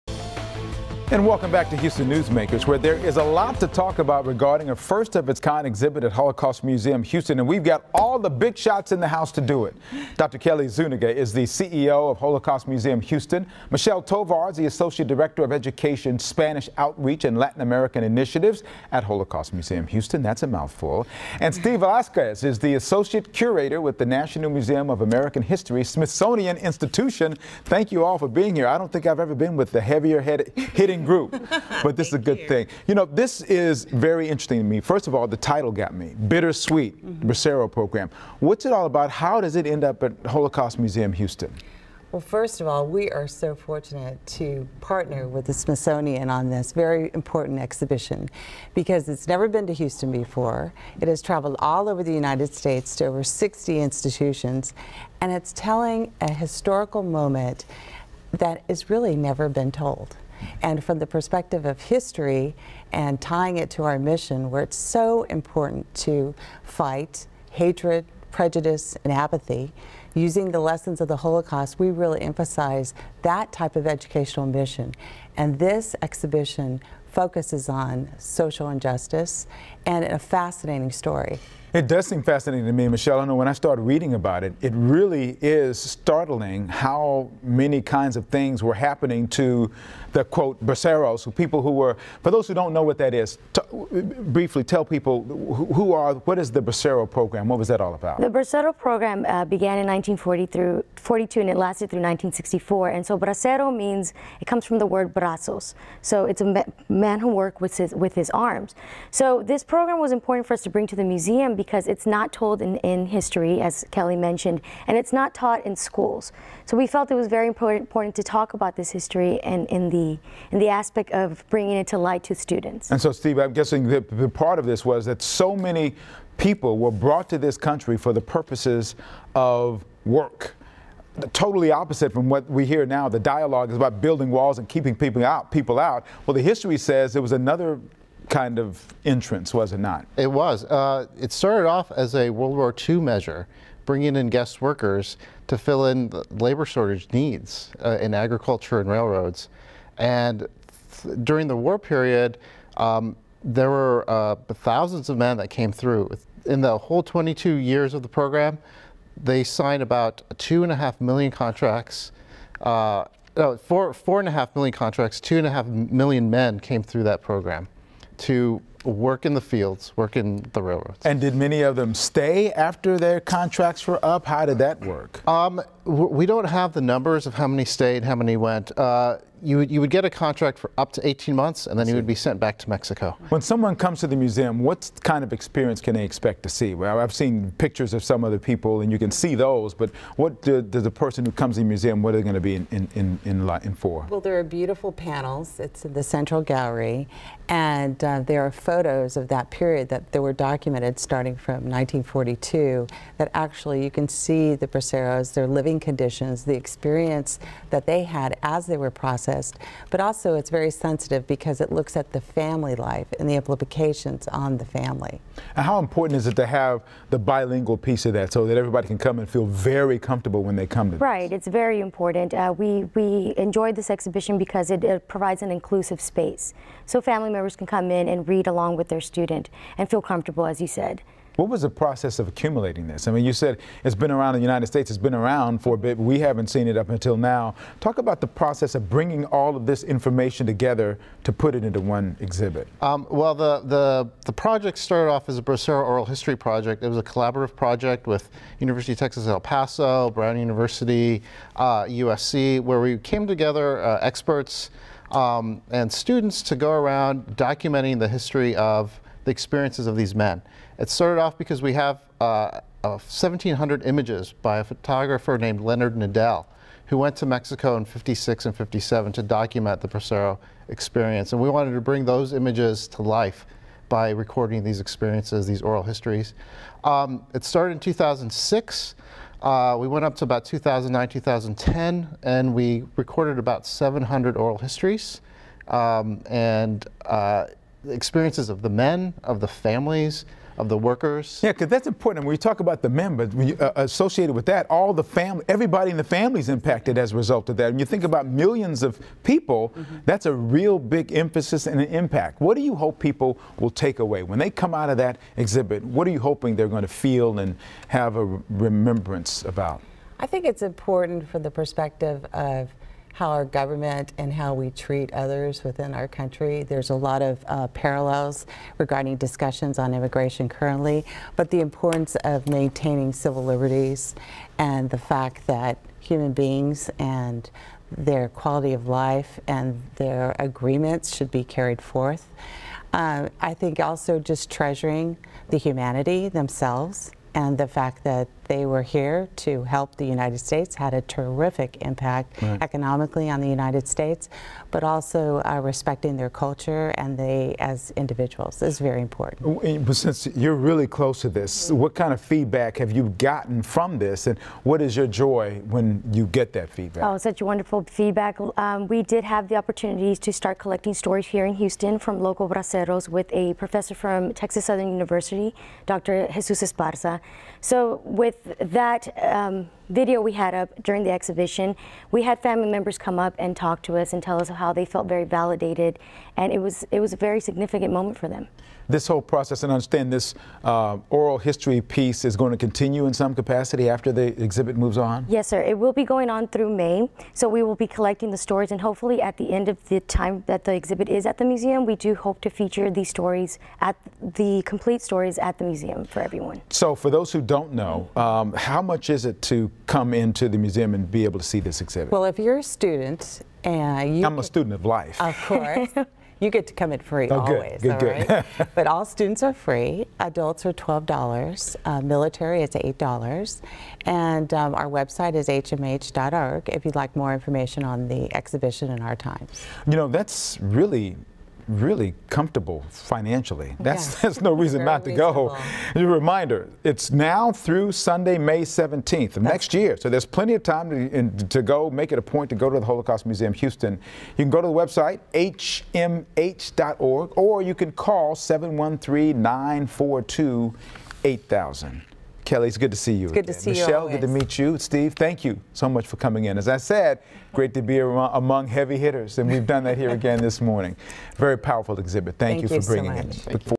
Newsmakers: Bracero Program exhibit in Houston
The Bracero Program exhibit at the Holocaust Museum Houston tells the story of Mexicans recruited to come to the United States to perform cheap labor.